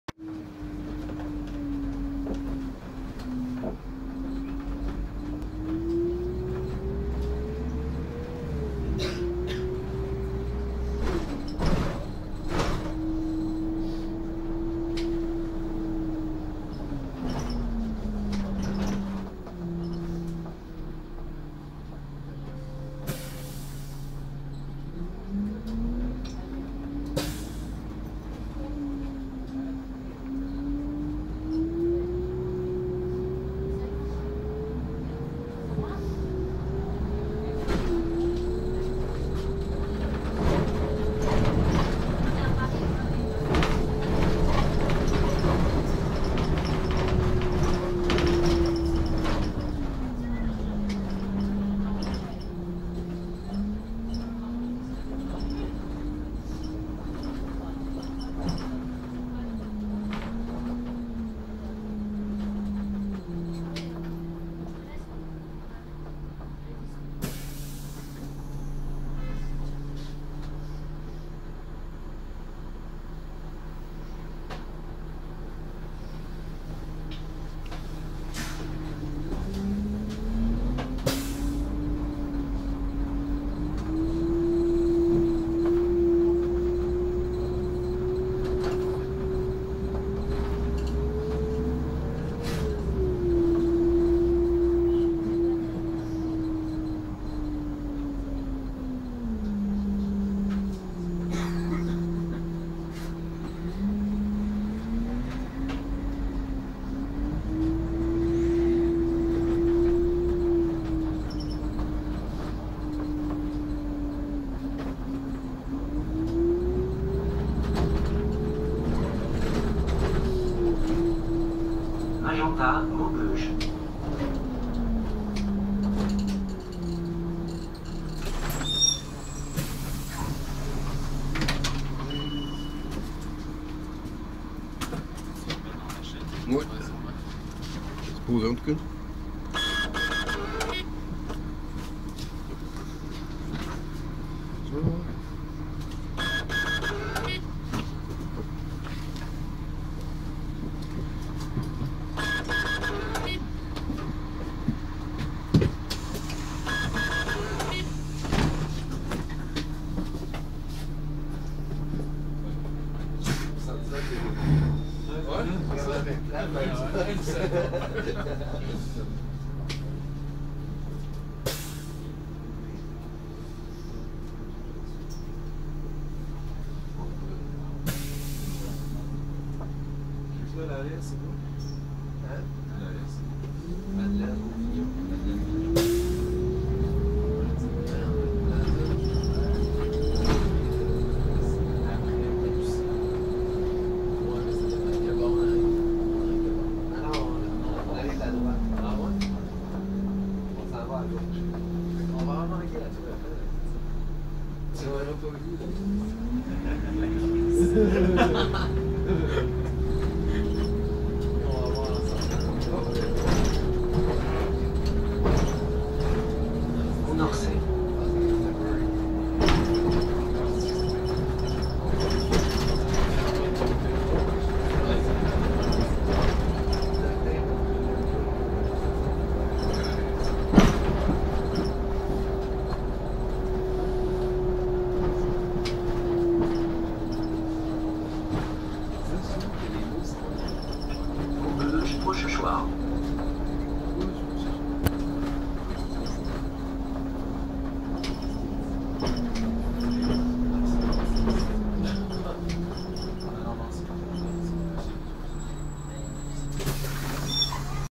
Agora V2 de la RATP sur la ligne 42 (2).
Renault Agora V2 de la RATP sur la ligne 42 entre Gare du Nord et Maubeuge – Rochechouart (Paris). Bus numéro: 7747 (Aout 2001). Direction: Hôpital Européen Georges Pompidou.